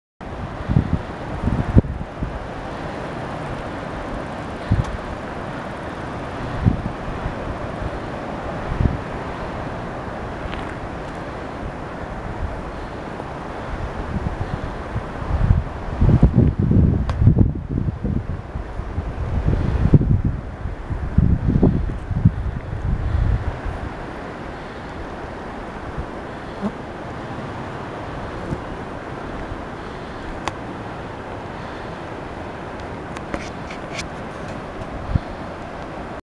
Thy - Woods
Woods in the sourthern part of the danish national park in Thy.